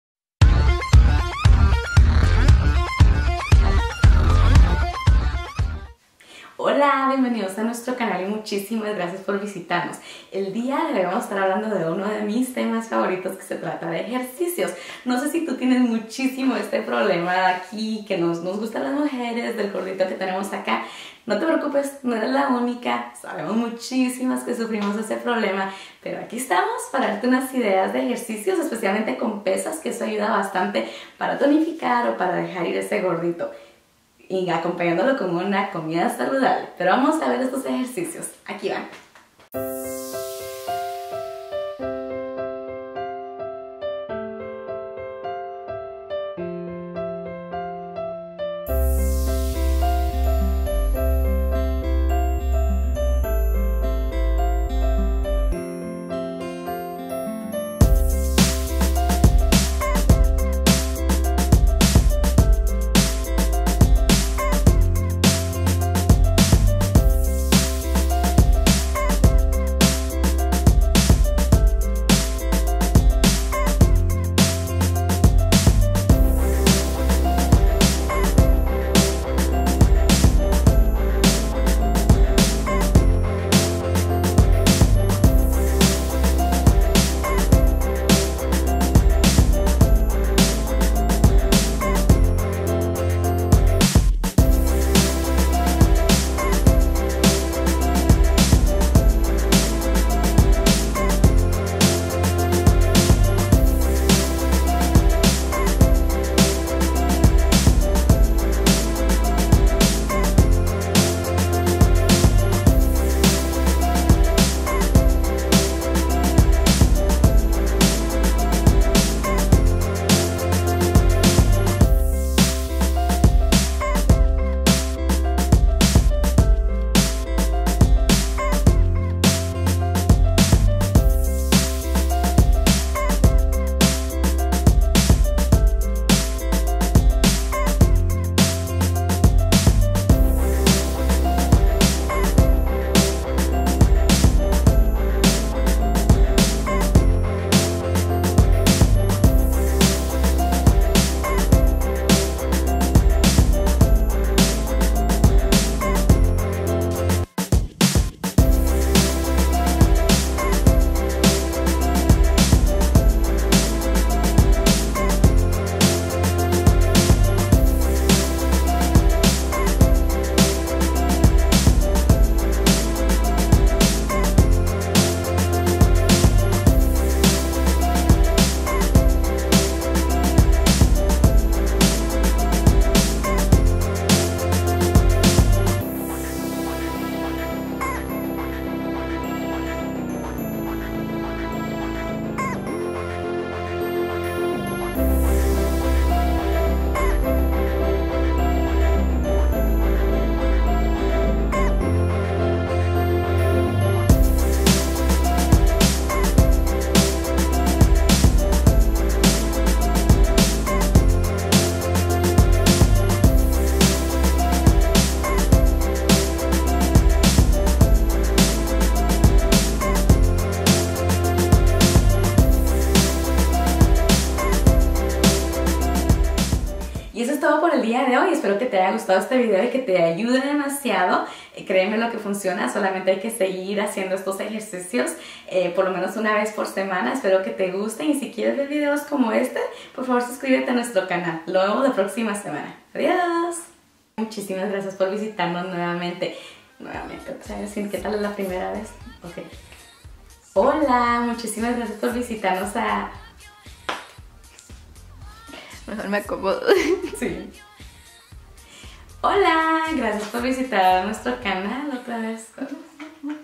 Ejercicios para Triceps
Hola!

Aquí les traigo una rutina de ejercicios para triceps. Espero la puedan seguir y les ayude a tonificar.

Graciela